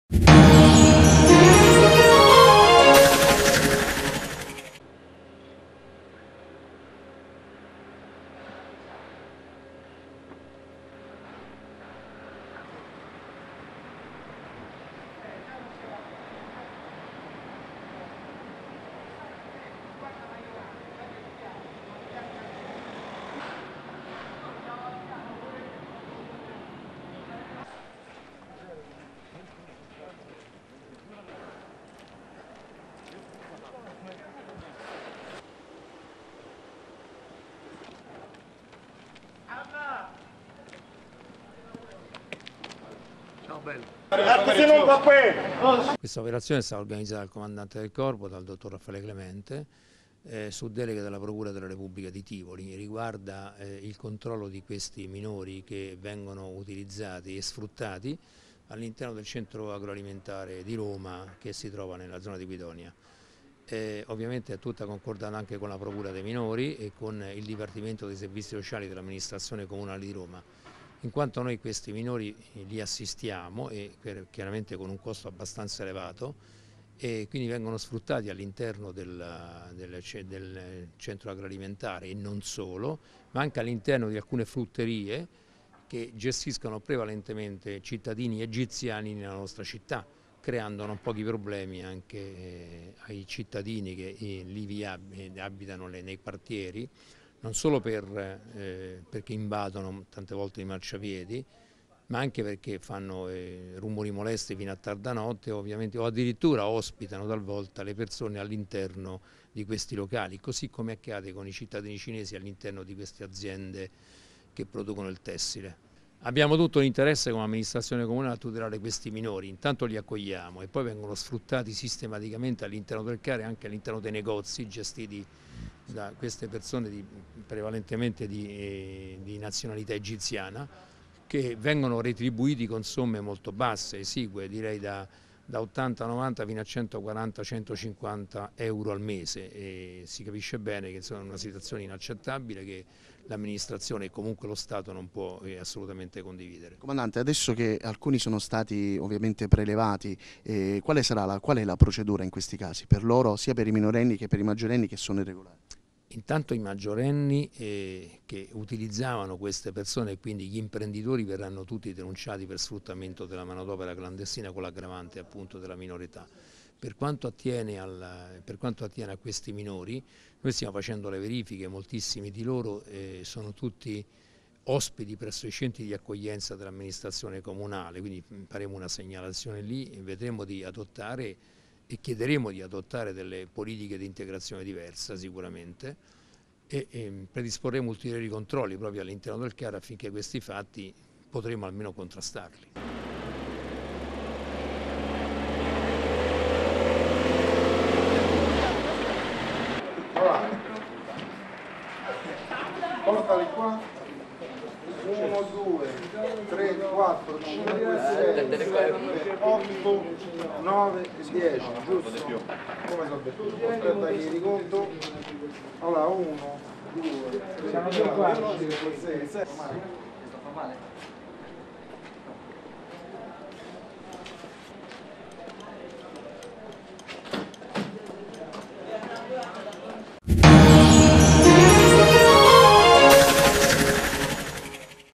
Sfruttamento minorile al Car, di notte accolti dal Campidoglio e di giorno lavoratori in nero
E' iniziata questa mattina una vasta operazione di contrasto  all' impiego di minori nel cosiddetto "lavoro nero"  presso il centro agroalimentare ed ittico di Roma, situato nel Comune di Guidonia Montecelio. L'operazione, inserita in una complessa indagine avviata nei mesi scorsi dal Corpo di Polizia Roma Capitale e delegata dalla Procura della Repubblica di Tivoli, ha permesso agli agenti dell' U.O.  Sicurezza Pubblica ed Emergenziale, di suffragare, quanto ipotizzato nel corso della precedente attivita' investigativa.